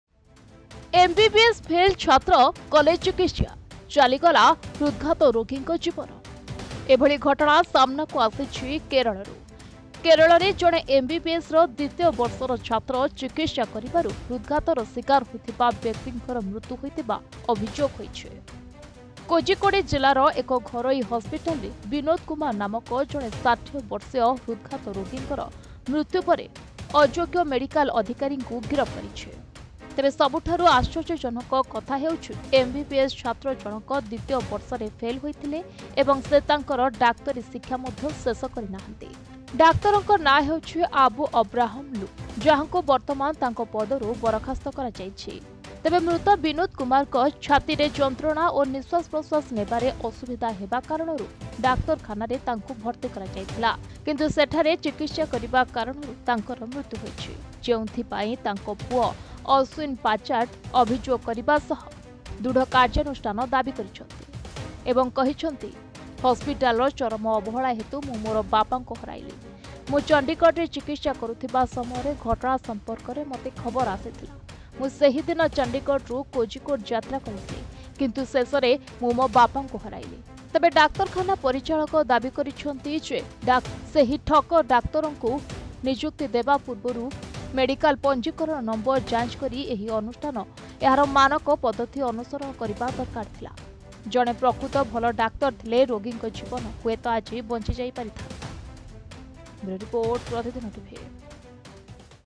MBBS ଫେଲ୍ ଛାତ୍ର କଲେ ଅପରେସନ।Heart patient in Kerala dies after treatment by failed MBBS student.
MBBS ଫେଲ୍ ଛାତ୍ର କଲେ ଅପରେସନ।Heart patient in Kerala dies after treatment by failed 2nd year MBBS student.